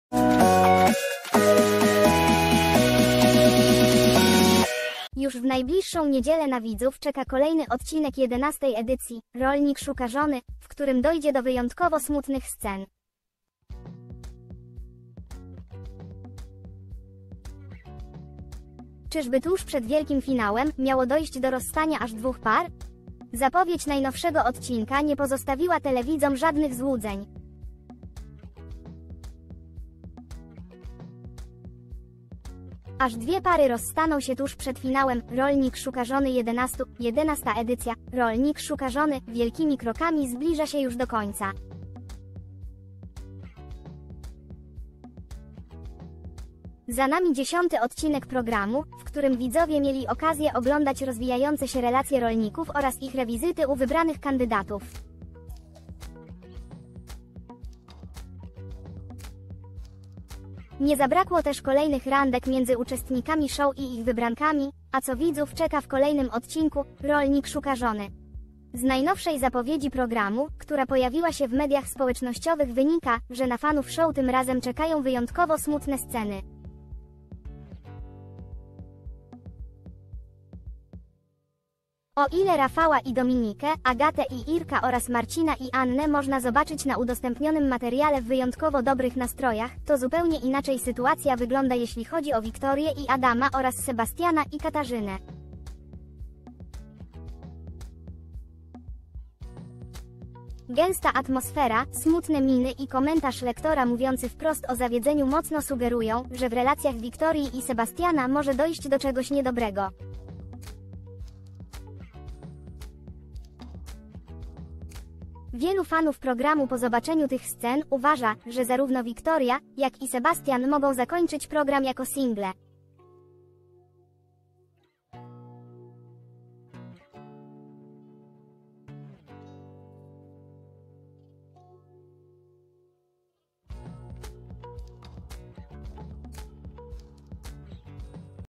„Rolnik szuka żony 11”. Aż dwie pary rozstaną się tuż przed finałem!? Wyjątkowo smutne sceny w kolej
Już w najbliższą niedzielę na widzów czeka kolejny odcinek 11. edycji „Rolnik szuka żony”, w którym dojdzie do wyjątkowo smutnych scen. Czyżby tuż przed wielkim finałem, miało dojść do rozstania aż dwóch par? Zapowiedź najnowszego odcinka nie pozostawiła telewidzom żadnych złudzeń.
Aż dwie pary rozstaną się tuż przed finałem „Rolnik szuka żony 11”?
11. edycja „Rolnik szuka żony” wielkimi krokami zbliża się już do końca. Za nami 10. odcinek programu, w którym widzowie mieli okazję oglądać rozwija  ...